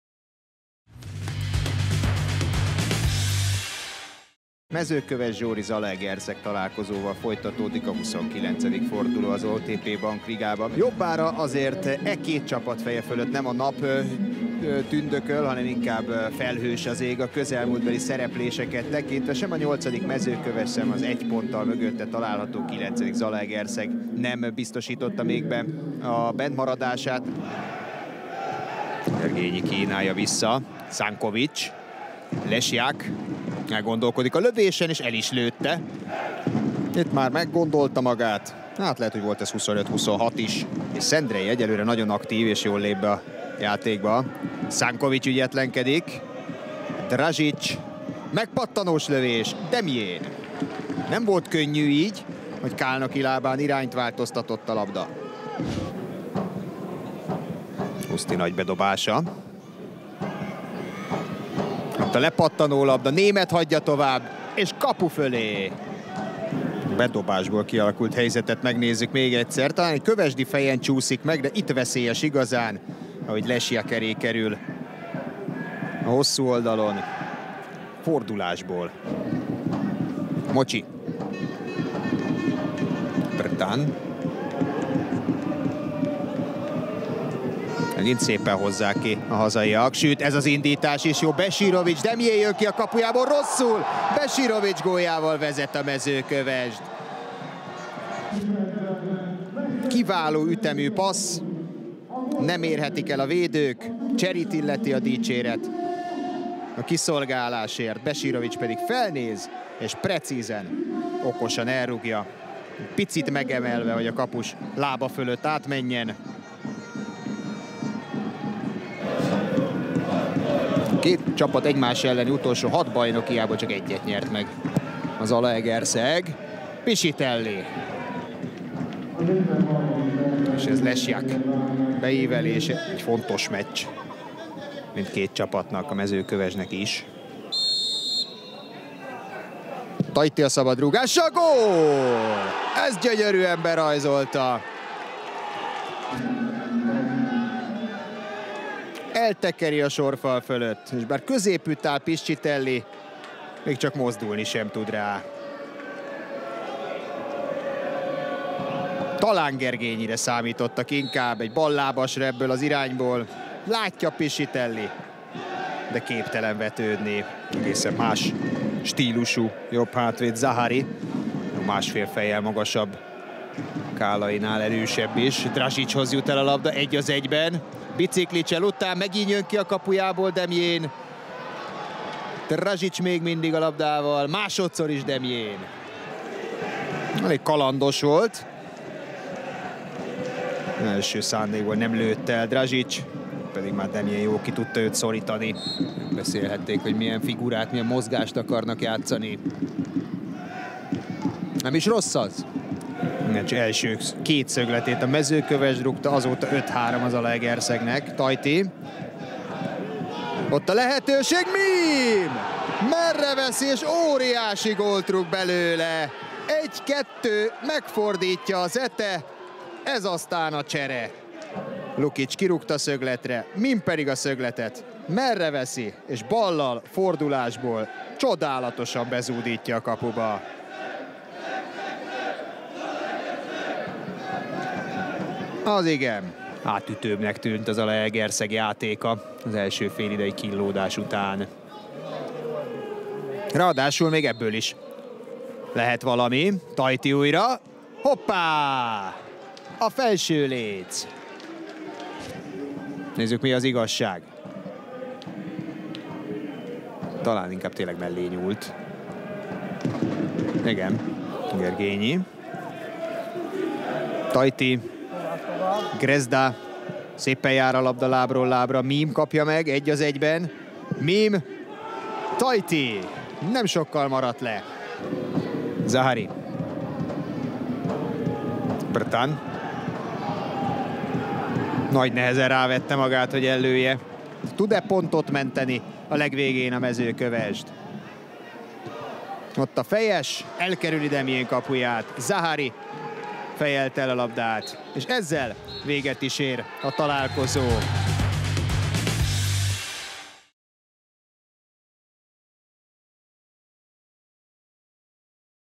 Mezőkövesd Zsóry FC - ZTE FC | 1-2 | (1-0) | OTP Bank Liga | 29. forduló | MLSZTV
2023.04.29. OTP Bank Liga, 29. forduló
Mezőkövesd Zsóry FC - ZTE FC | 1-2 | (1-0) |
összefoglaló